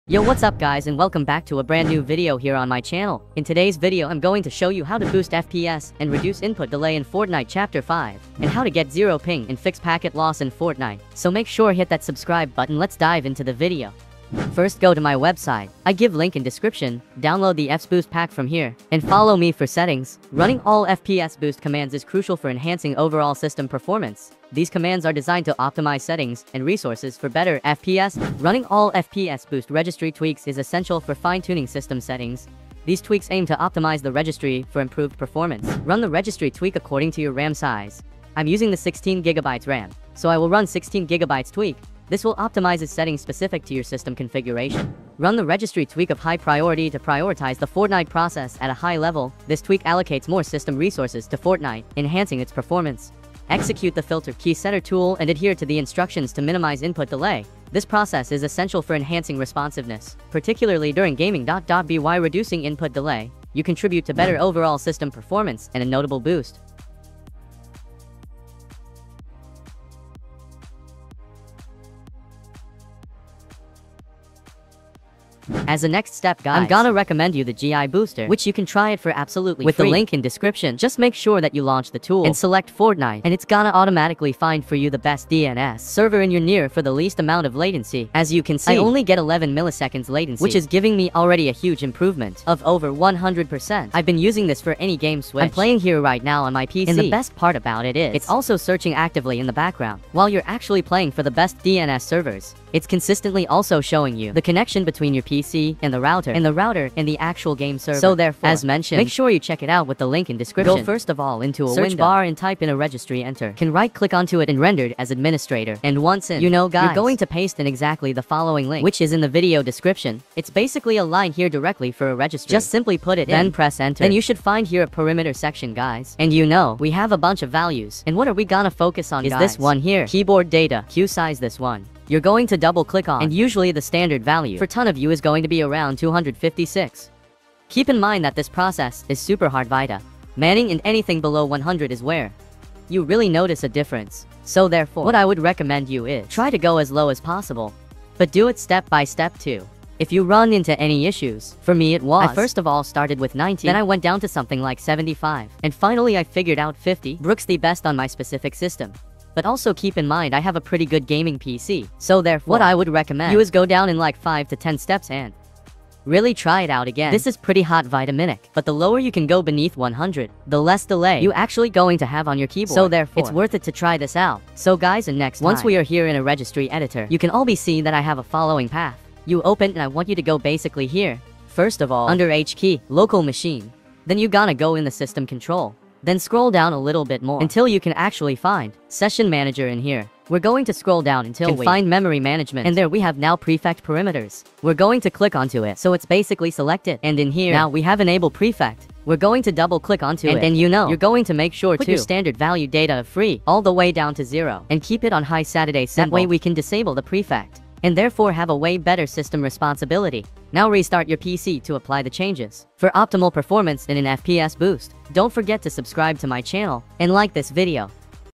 Use These Settings To Boost FPS & Lower Input Delay in Fortnite Chapter 5!
In this video, I'm going to show you the secret Registry Settings For BOOST FPS & FIX STUTTERS in Fortnite. FPS drops are a common problem for Fortnite players, and this video will show you a fix that's guaranteed to work.

By following these simple steps, you'll be able to get more FPS and stutter-free gameplay in Fortnite! This video is for Fortnite only, so make sure you're using all the Tweaks and Best Settings for MAX FPS Boost & Less Input Delay

Title: Use These Settings To Boost FPS & Lower Input Delay in Fortnite Chapter 5!